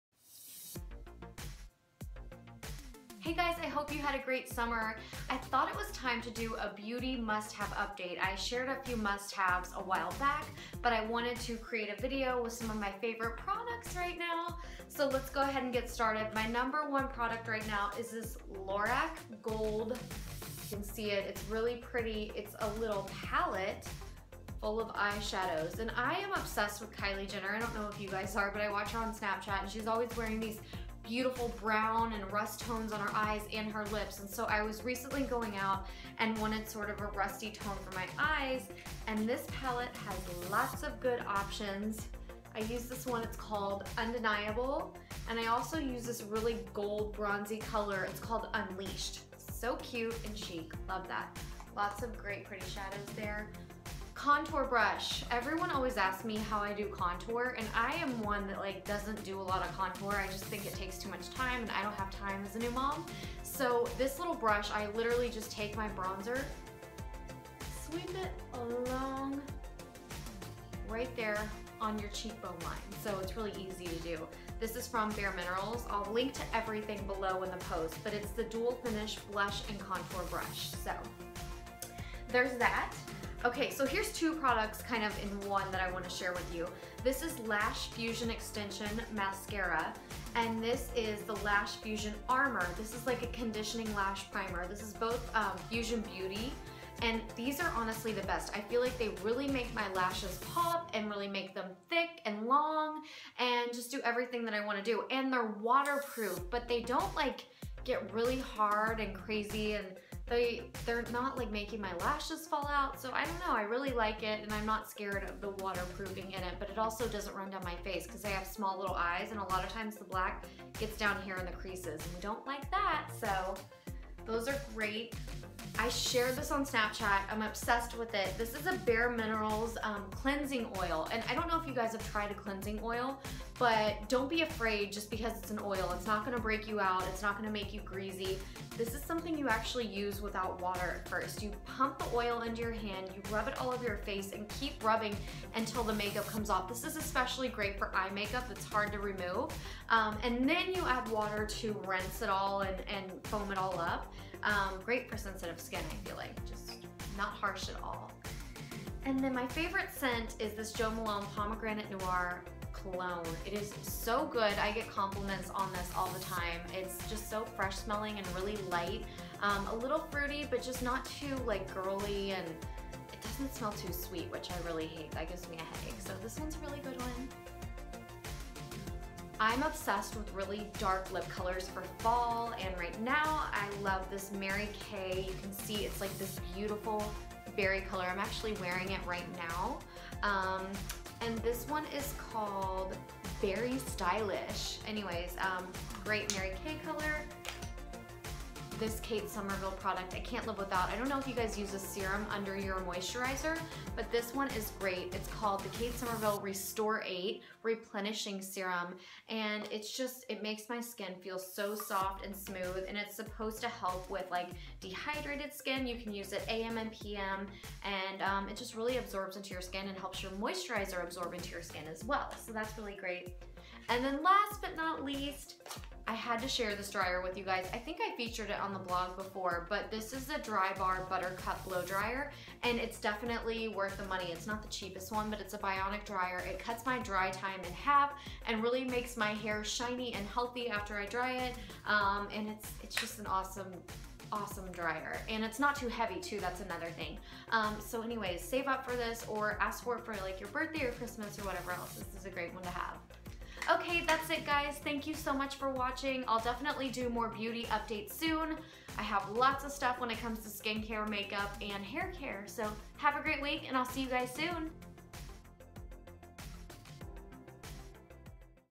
Best Beauty Products Sept 2016
Sharing the the best and my favorite new beauty products for September 2016.
Fashion Blogger and Style Expert Ashley Fultz from TheStyleEditrix.com shares her favorite new beauty products in makeup, skincare and haircare.  To shop all of the products go